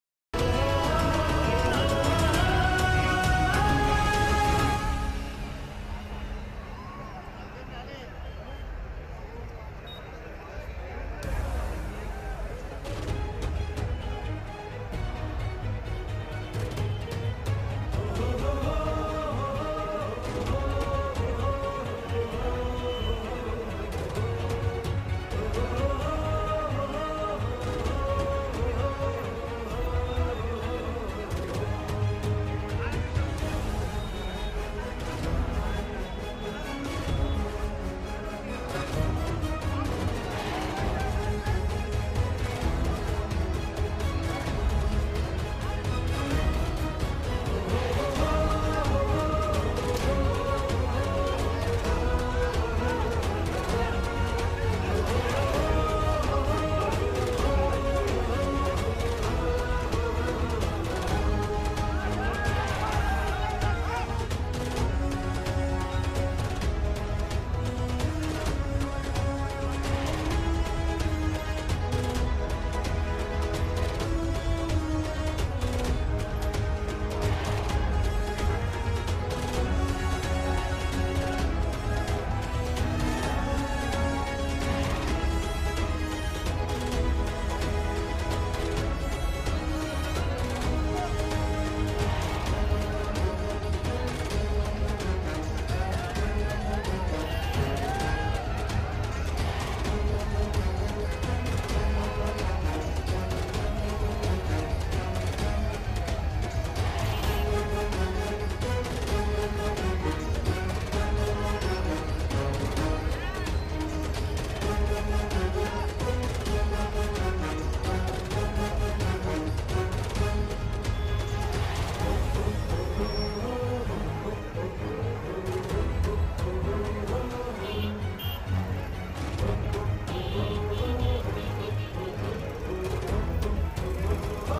rahulgandhi , अभिनेत्री पूजा भट्ट
bharatjodoyatra , rahulgandhi , telangana , पूजा भट्ट राहुल गांधी के साथ भारत जोड़ो यात्रा में शामिल ।